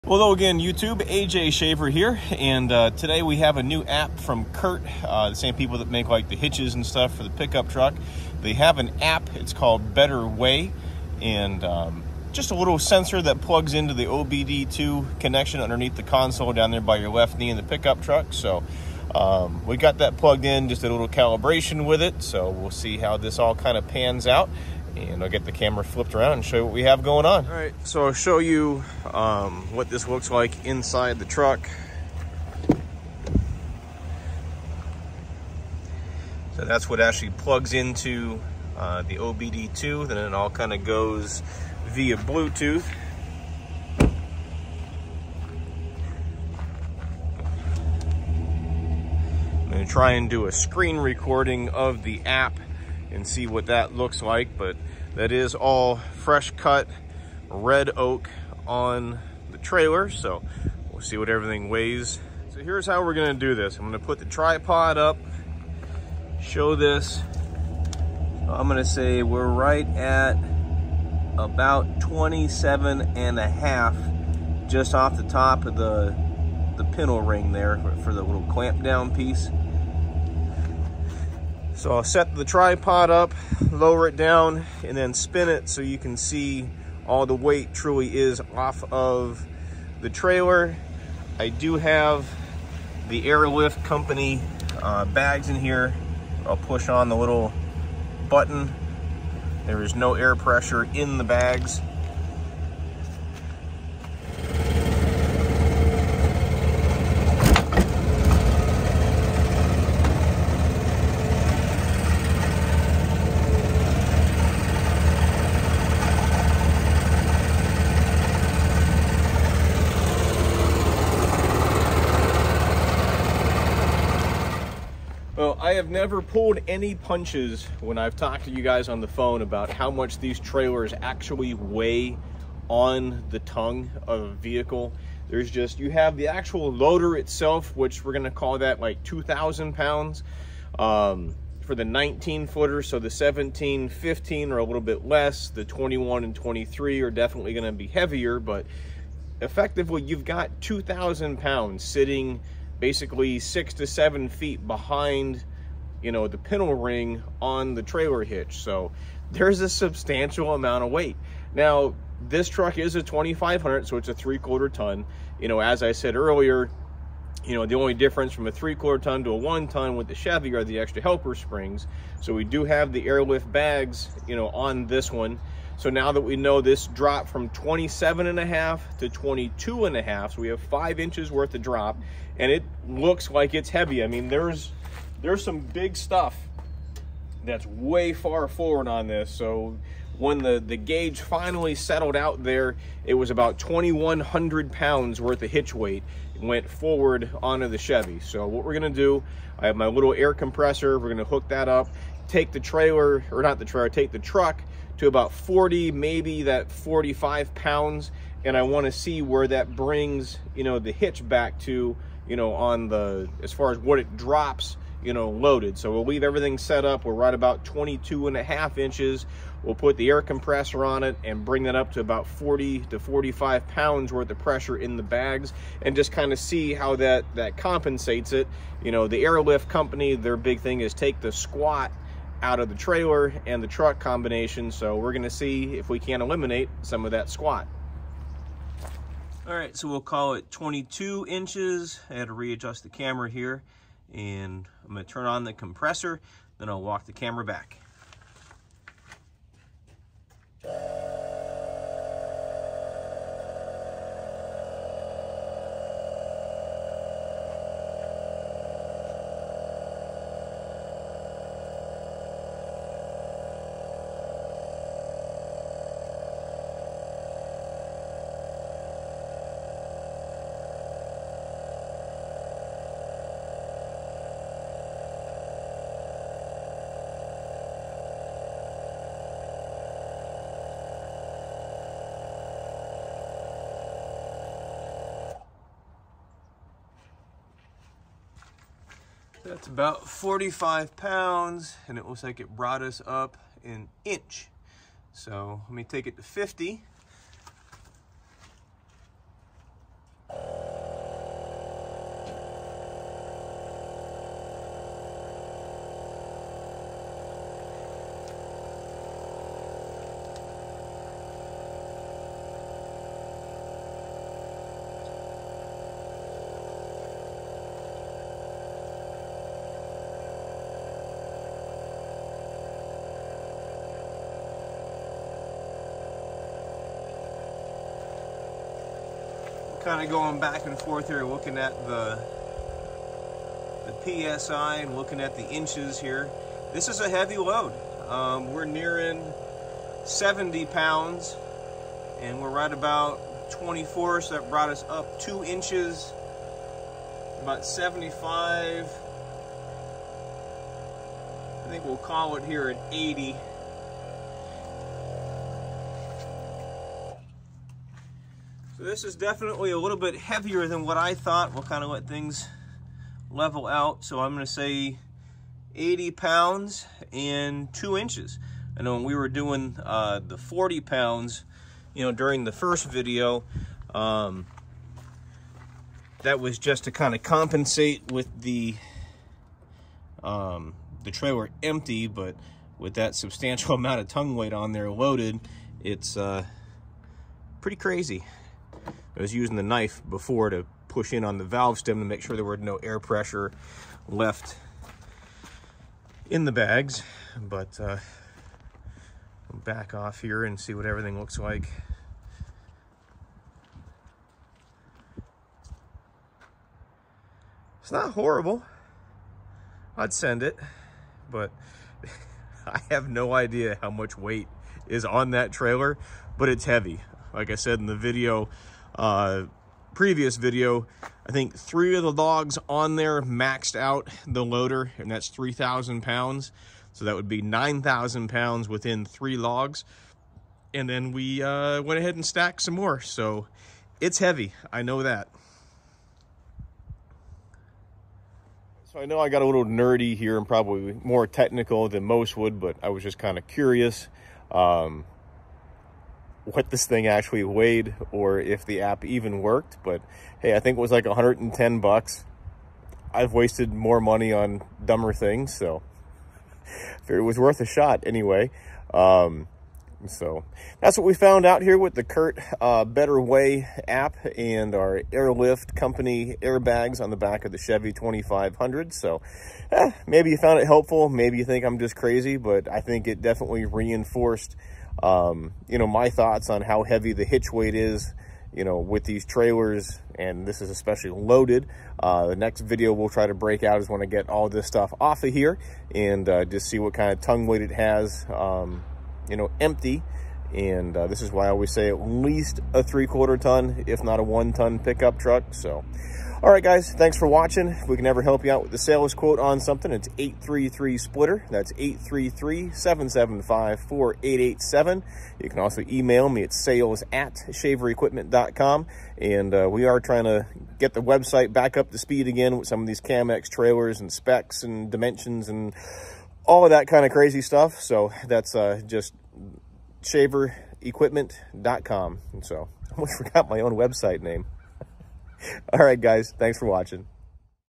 AirLift Co Air Bags LOADED HEAVY with Red Oak
Now that our @AirLiftCompany air bags are installed, let's see what they can do! Also, played around with Curt's "Better Weigh" new weight management app. After a quick calibration, we were able to figure out hitch weight on the GN, tongue weight on the bumper pull and even cargo weight in the bed. However, for this video we just focus in on the tongue weight with a very heavy load of Red Oak logs from my neighbor's house.

We don't get paid to do our videos. Our videos showcase what we sell and we'd love to sell directly to you!